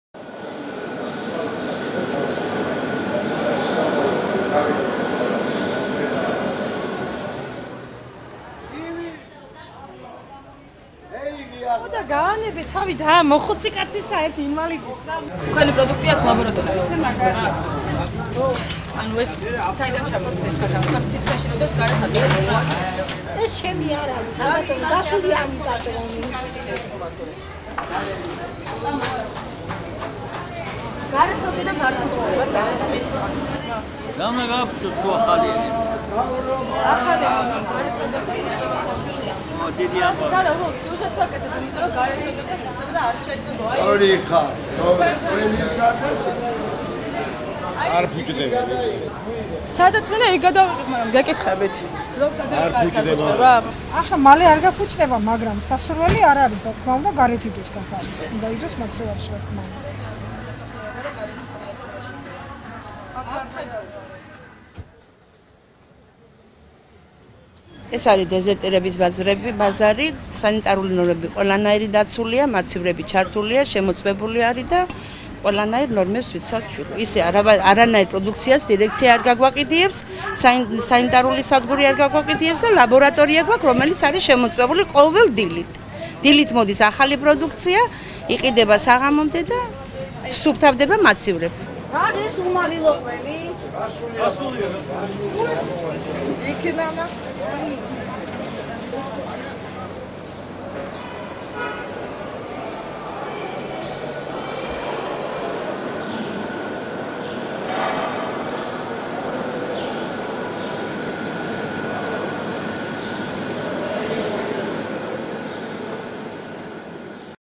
რა ხდება თბილისის ცენტრალურ ბაზარში
ზაფხულის დადგომასთან და ტემპერატურის მატებასთან ერთად, მოსახლეობას საკვები პროდუქტების შეძენისას განსაკუთრებული ყურადღება მართებს. იმის გასაგებად, თუ რა სიტუაციაა ე.წ. დეზერტირების ბაზარსა და მის მიმდებარე ტერიოტიაზე, PALITRATV.GE ბაზარში მოვაჭრეებს ესაუბრა. იხილეთ, როგორ ინახავენ გასაყიდ ხორცს და განსაზღვრეთ, რამდენად საშიში შეიძლება იყოს ხორცი მაცივრის გარეშე.